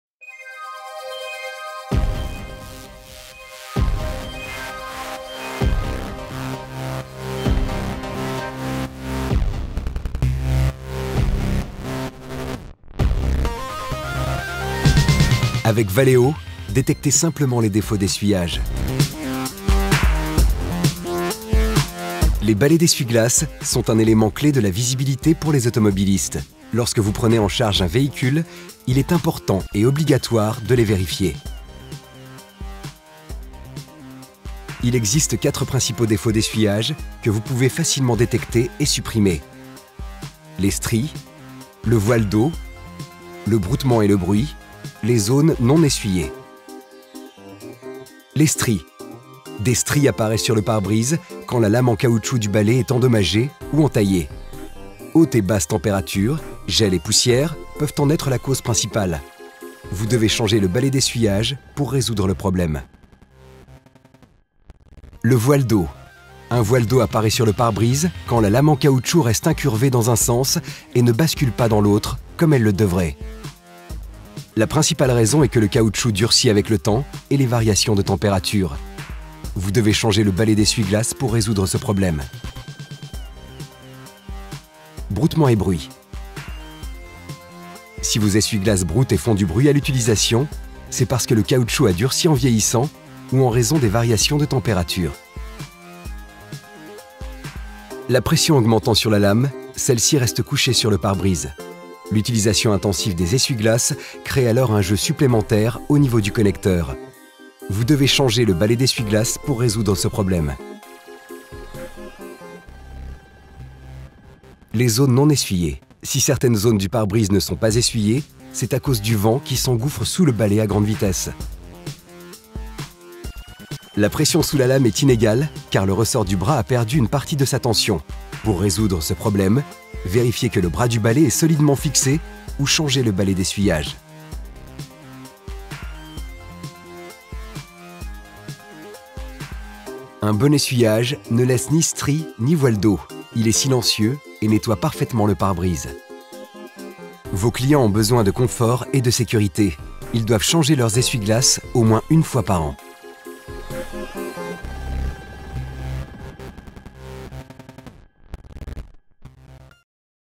[Driving Assistance] Avec Valeo, détectez simplement les défauts d'essuyage
Il existe 4 principaux défauts d'essuyage que vous pouvez facilement détecter et supprimer :

1. Les stries
Des stries apparaissent sur le pare-brise quand la lame en caoutchouc du balai est endommagée ou entaillée. Vous devez changer le balai d'essuyage pour résoudre le problème.

2. Le voile d'eau
Un voile d'eau apparait sur le pare-brise quand la lame en caoutchouc reste incurvée dans un sens et ne bascule pas dans l'autre, comme elle le devrait. Vous devez changer le balai d'essuyage pour résoudre le problème.

3. Broutement et bruit.
Si vos essuie-glaces broutent et font du bruit à l'utilisation, c'est parce que le caoutchouc a durci en vieillissant ou en raison des variations de température. Vous devez changer le balai d'essuyage pour résoudre le problème.

4. Les zones non essuyées.
Si certaines zones du pare-brise ne sont pas essuyées, c'est à cause du vent qui s'engouffre sous le balai à grande vitesse. Vous devez changer le balai d'essuyage pour résoudre le problème.

Un bon essuyage ne laisse ni strie, ni voile d'eau ; il est silencieux et nettoie parfaitement le pare-brise.

Valeo, la technologie automobile, naturellement.